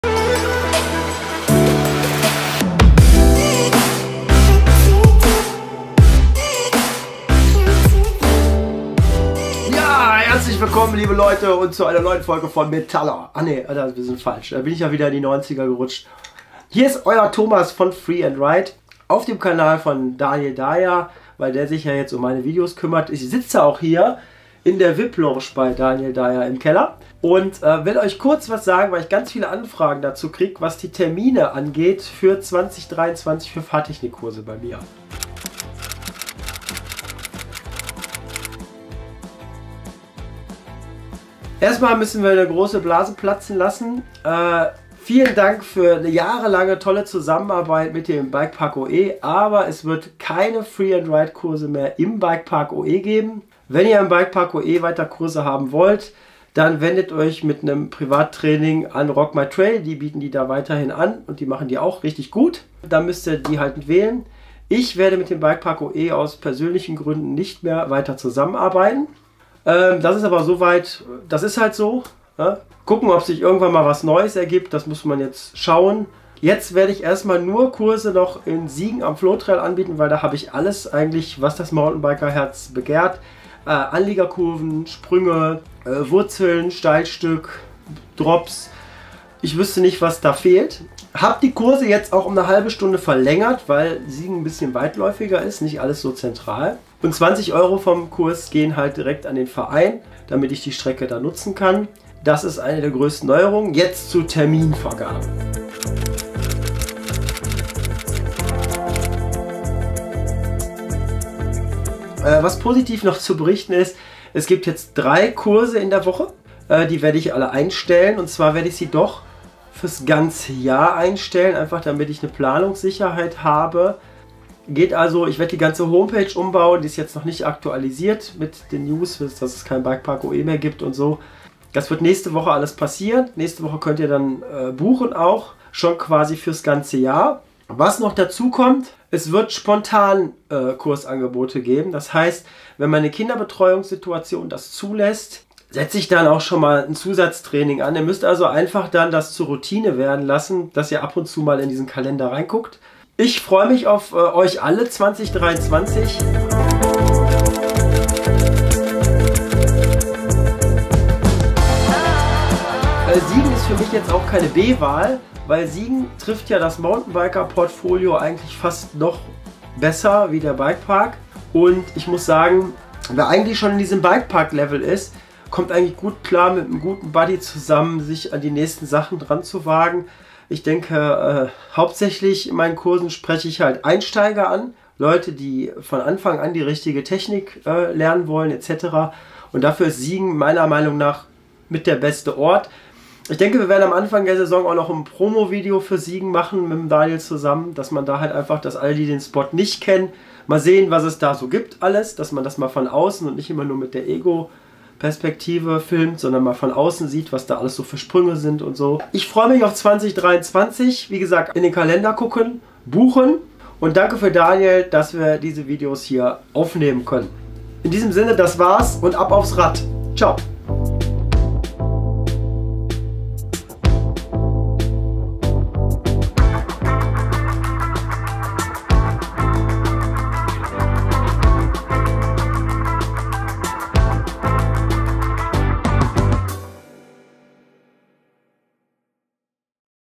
NEWS Free and Ride 2023 - Mountainbike Fahrtechnik Kurse in NRW
Es gibt News für die Free and Ride Mountainbike Fahrtechnik Kurse in NRW von Tomas.

►►► Meine Bike Versicherung auch für Downhill ( DAYA7 für einen Monat geschenkt)

►►►  MTB Ausrüstung